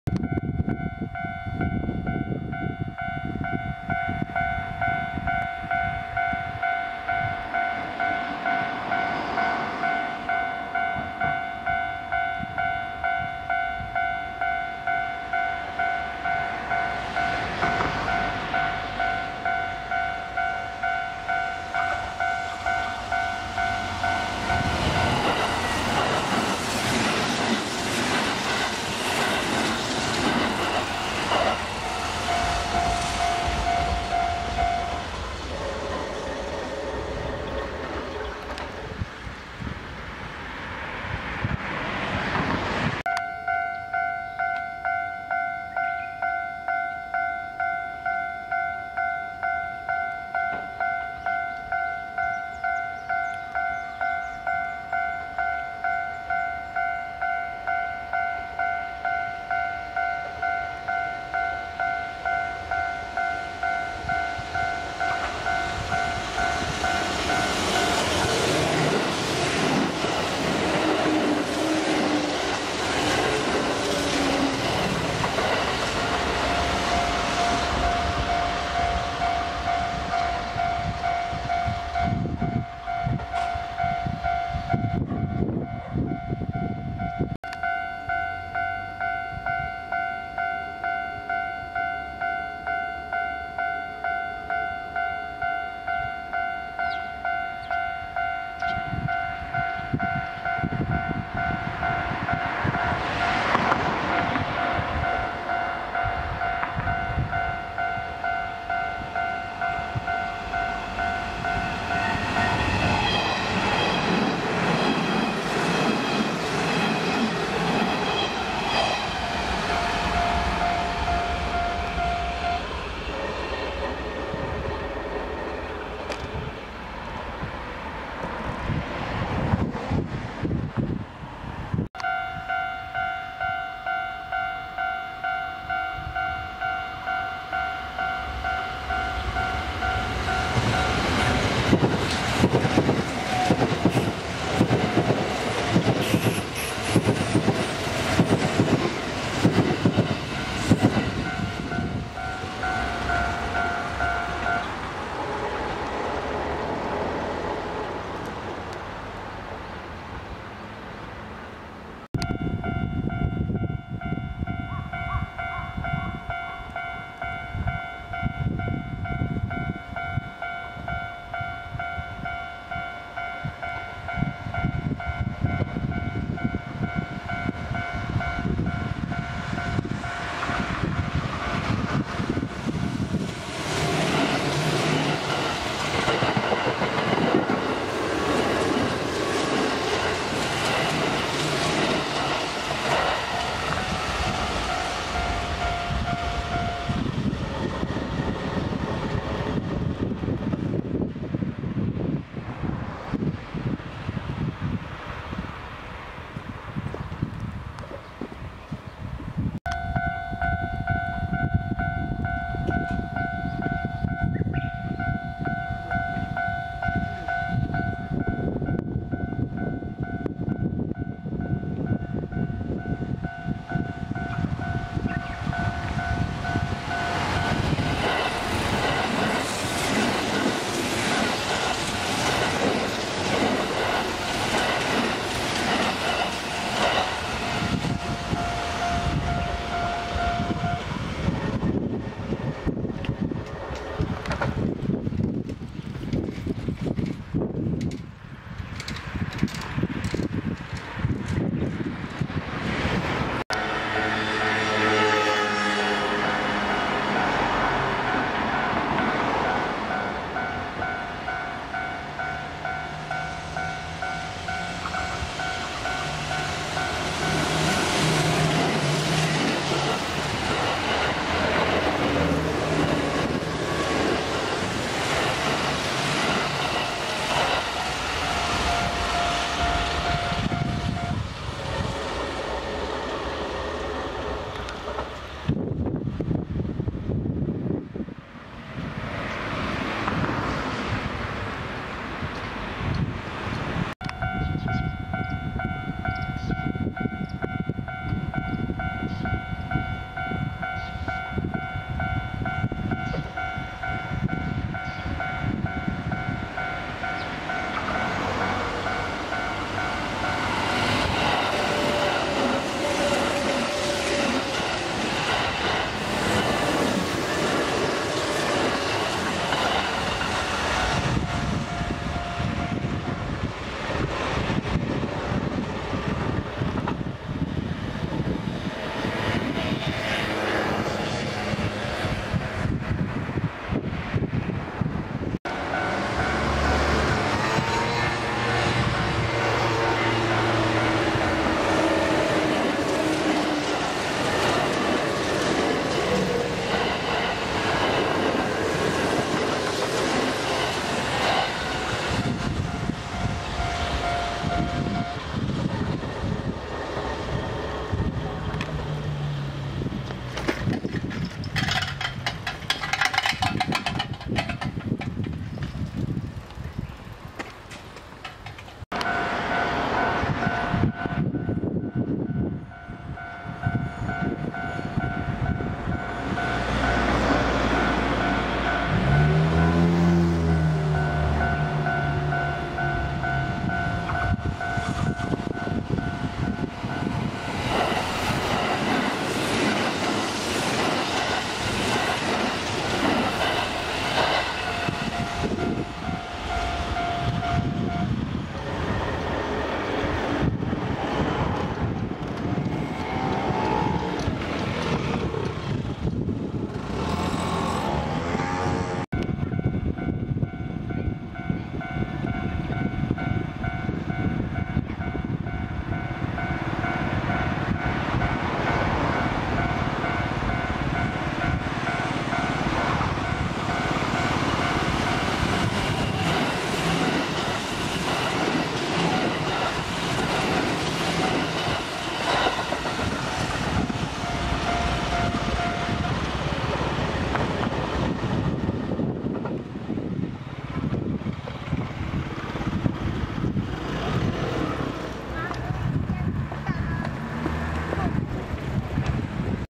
近鉄奈良線 平日朝ラッシュ時の6・8両準急 動画集
近鉄奈良線、朝ラッシュ時の6・8両の大阪難波・尼崎・奈良行き準急を集めた動画です。
学園前～富雄間の踏切外で撮影しています。